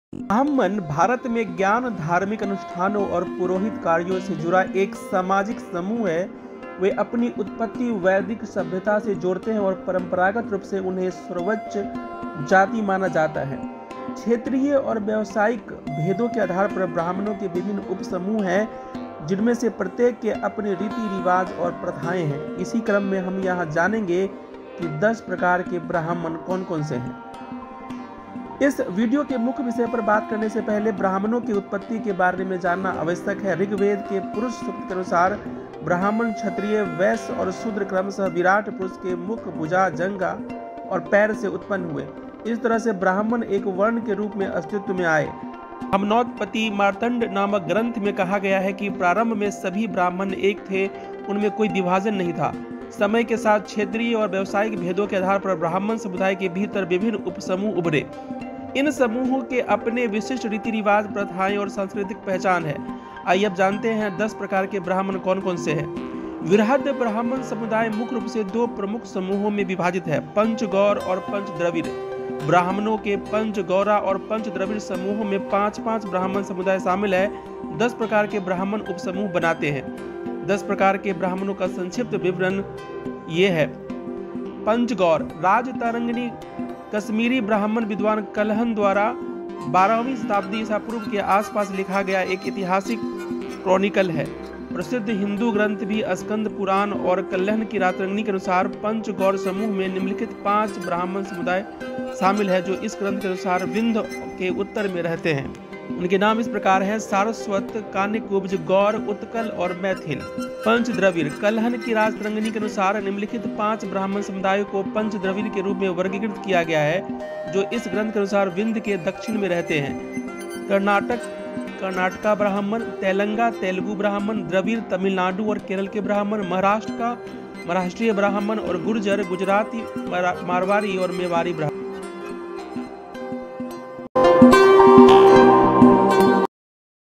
10 प्रकार के ब्राह्मण कौन-कौन से हैं? What are the ten types of Brahmins?
ब्राह्मण भारत में ज्ञान, धार्मिक अनुष्ठानों और पुरोहित कार्यों से जुड़ा एक सामाजिक समूह है. वे अपनी उत्पत्ति वैदिक सभ्यता से जोड़ते हैं और परंपरागत रूप से उन्हें सर्वोच्च जाति माना जाता है. क्षेत्रीय और व्यावसायिक भेदों के आधार पर ब्राह्मणों के विभिन्न उपसमूह हैं, जिनमें से प्रत्येक के अपने रीति-रिवाज और प्रथाएं हैं इसी क्रम में यहां हम जानेंगे कि दस प्रकार के ब्राह्मण कौन-कौन से हैं. @castehistory

इस वीडियो में है:
10 प्रकार के ब्राह्मण कौन-कौन से हैं? What are the ten types of Brahmins?